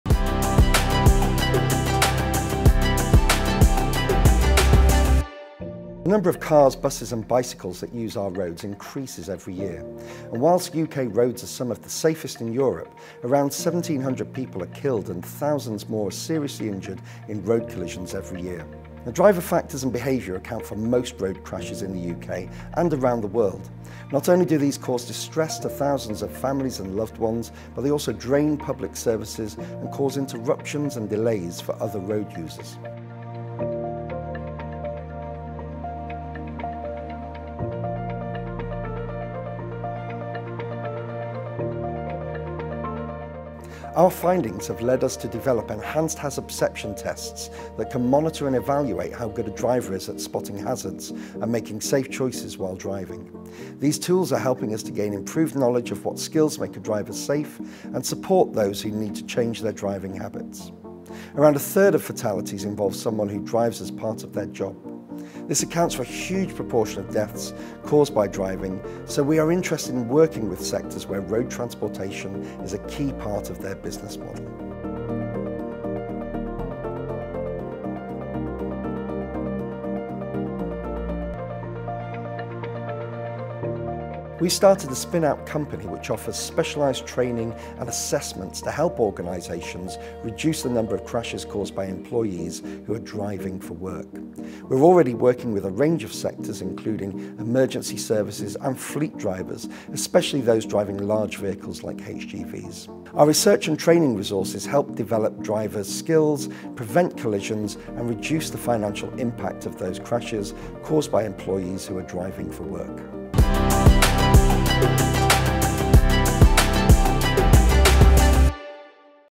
Re:drive: Reducing risk on our roads
🚗 Even though UK roads are among the safest in the world, 1,700 people die and thousands more are seriously injured on them every year.

NTU researcher, Professor David Crundall is improving the safety of UK roads by helping drivers to predict potential hazards, particularly those who work in industries where driving is part of their job.